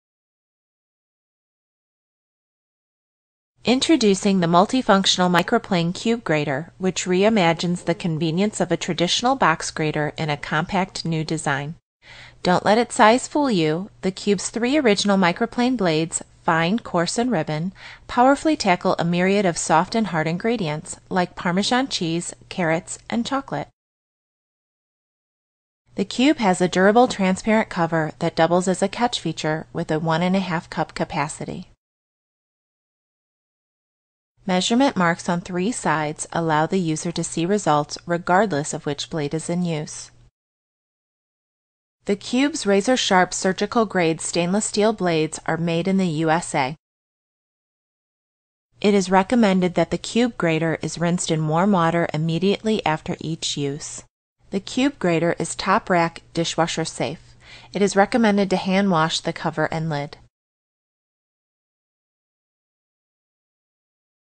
Cube Grater râpe boîtier 3 faces sur attitudesnews
Compact et peu encombrant, un ustensile pour râper et zester avec compartiment de conservation, nouveau sur attitudesnews.fr